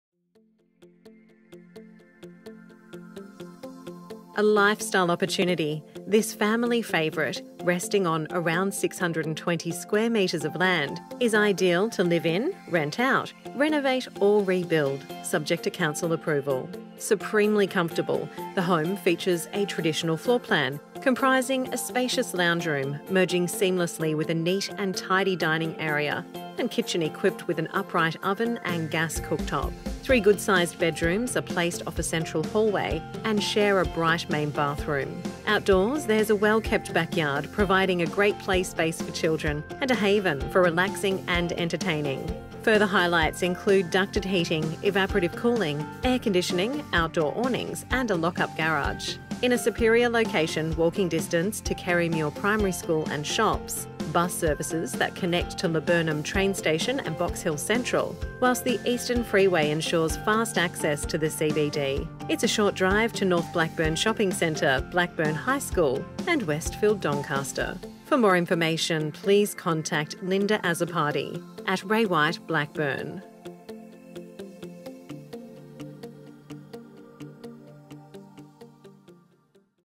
81 Heathfield Rise, Box Hill North
A lifestyle opportunity; this family favourite resting on 620sqm approx. of land is ideal to live in, rent out, renovate or rebuild (STCA). Supremely comfortable, the home features a traditional floorplan comprising a spacious north facing lounge room; merging seamlessly with a neat and tidy dining area and kitchen equipped with an upright oven and gas cooktop. Three good size bedrooms are placed off a central hallway and share a bright main bathroom. Outdoors there's a well-kept backyard with an expanse of soft grass providing a great play space for children and a haven for relaxing and entertaining. Further highlights include ducted heating, evaporative cooling, air conditioning, outdoor awnings and a lock up garage. In a superior location walking distance to Kerrimuir Primary School and shops, bus services that connect to Laburnum Train Station and Box Hill Central; whilst the Eastern Freeway ensures fast access to the CBD. It's a short drive to North Blackburn Shopping Centre, Blackburn High School and Westfield Doncaster.